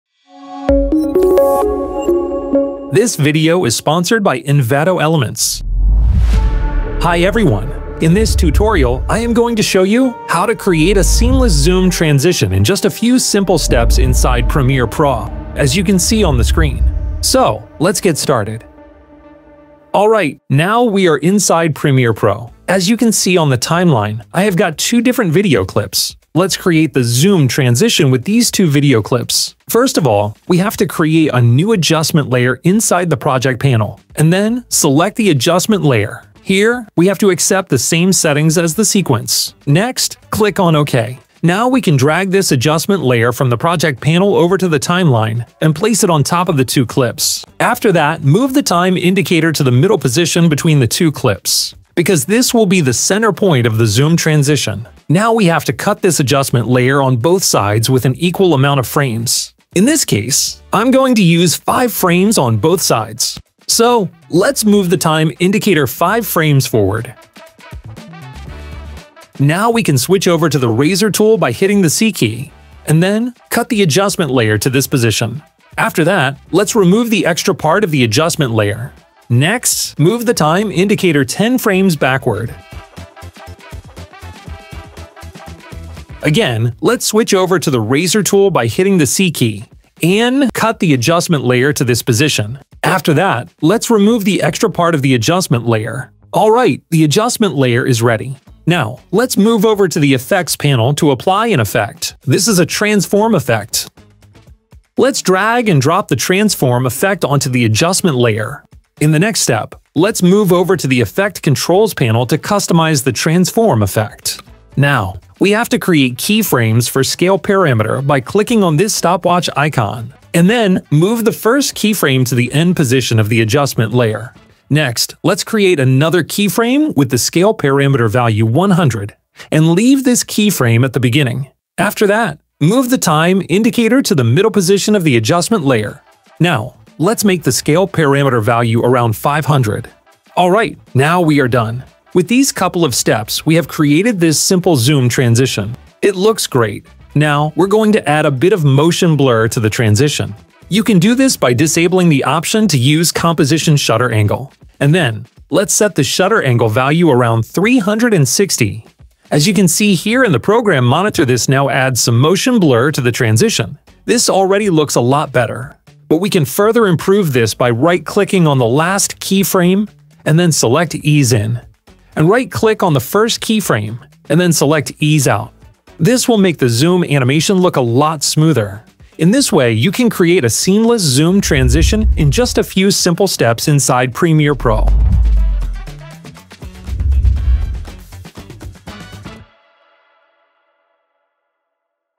Create ZOOM Transition in Premiere Pro
($50 OFF)
Learn how to incorporate a captivating and visually engaging ZOOM transition into your Premiere Pro projects. This tutorial is suitable for both novice and seasoned editors, and it will elevate the quality of your videos, making them more visually appealing and professional.